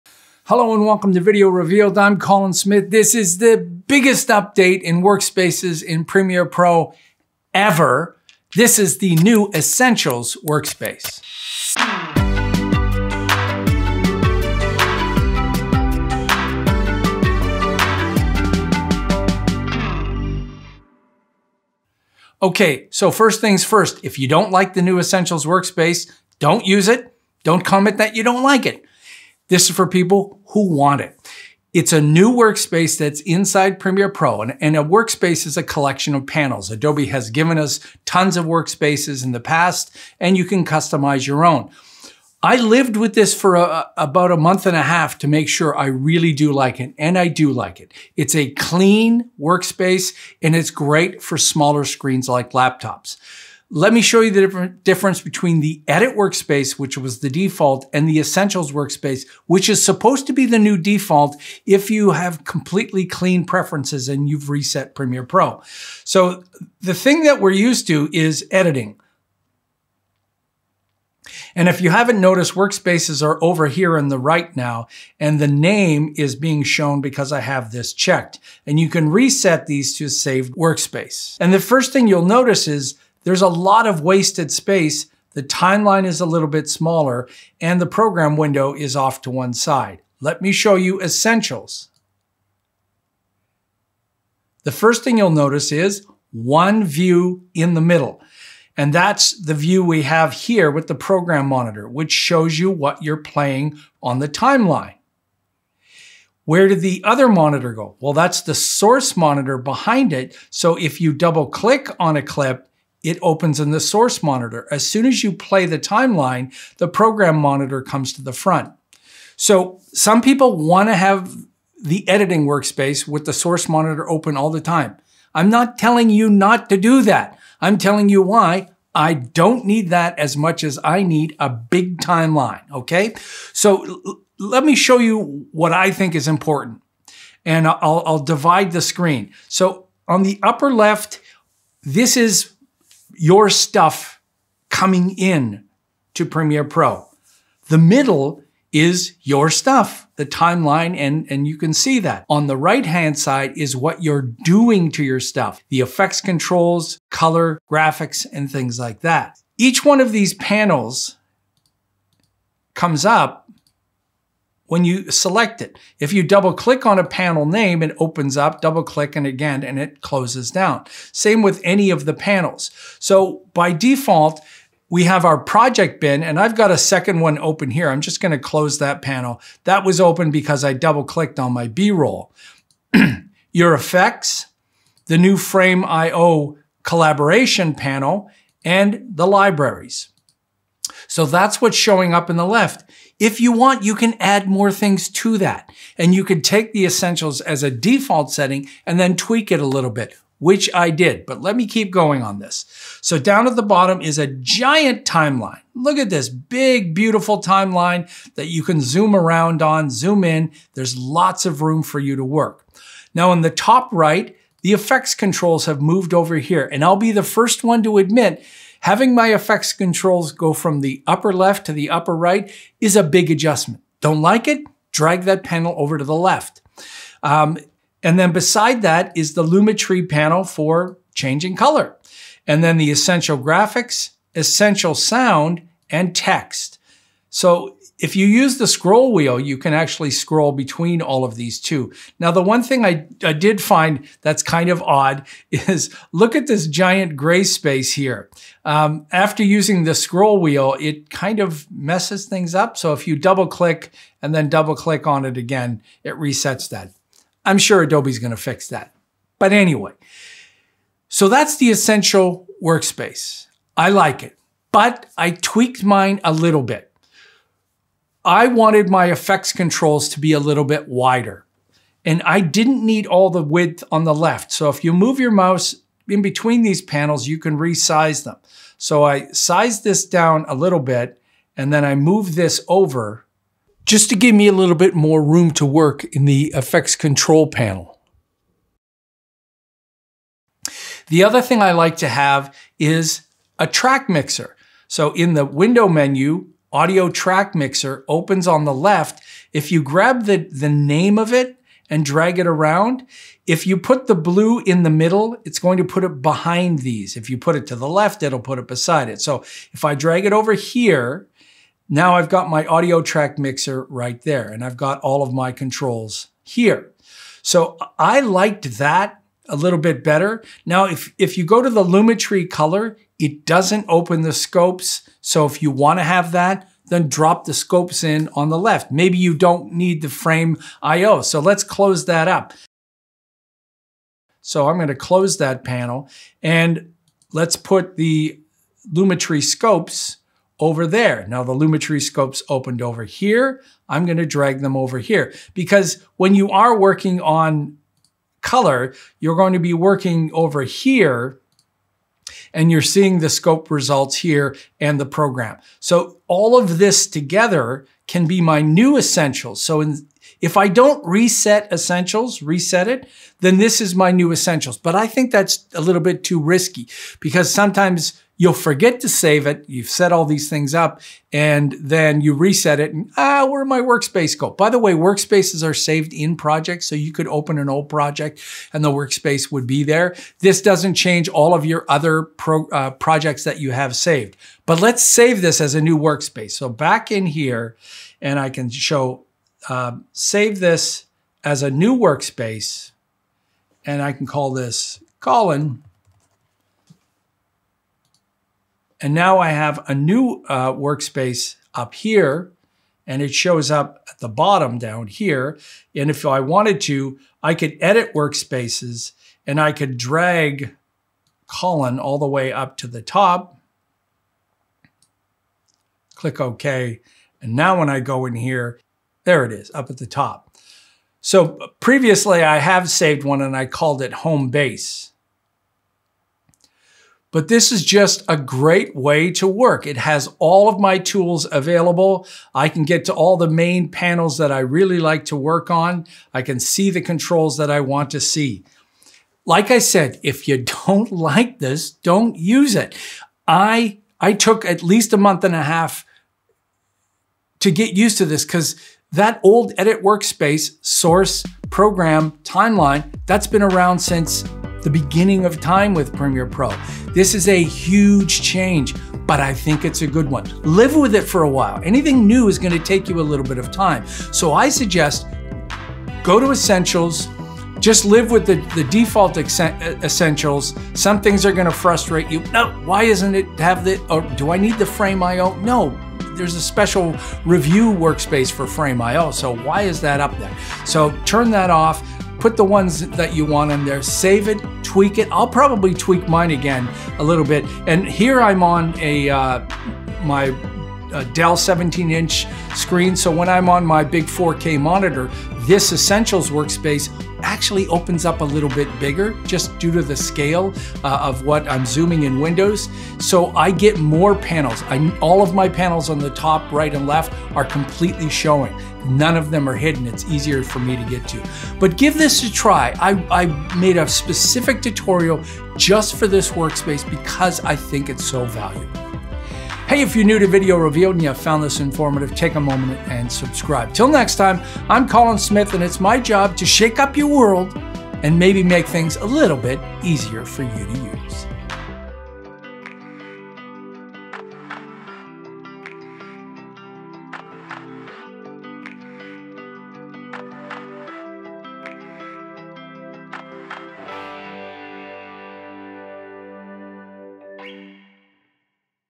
The BIGGEST update ever for Workspaces in Premiere Pro
This tutorial shows you the new Essentials Workspace and why it's very useful. It's great for get the most room on a laptop but also useful for large screen monitors. It also includes useful tips for creating new Workspaces and tweaking existing Workspaces

QUESTIONS?

THANKS!